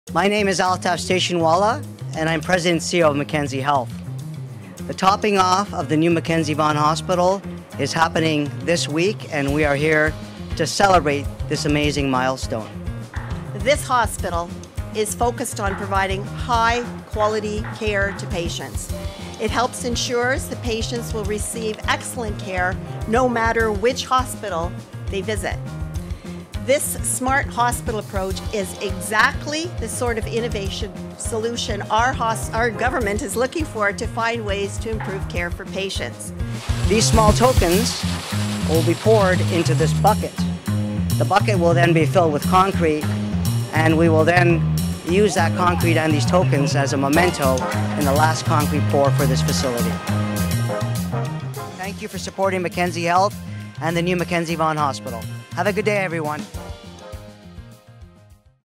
Cortellucci Vaughan Hospital - We’re pouring the top floor! (summary video)
We’re excited to announce that we’ve poured one of the final concrete slabs at the top of  Cortellucci Vaughan Hospital – 11 stories high and hundreds of feet in the air. At yesterday’s official topping off ceremony, we were joined by the Honourable Christine Elliott, Deputy Premier and Minister of Health and Long-Term Care; Stephen Lecce, MPP for King-Vaughan, Parliamentary Assistant to the Premier and Parliamentary Assistant to the Minister of Infrastructure; the Honourable Michael Tibollo, MPP Vaughan-Woodbridge, Minister of Community Safety and Correctional Services; and the Honourable Maurizio Bevilacqua, Mayor of Vaughan, to celebrate reaching the highest point of construction and getting closer to opening our doors in late 2020.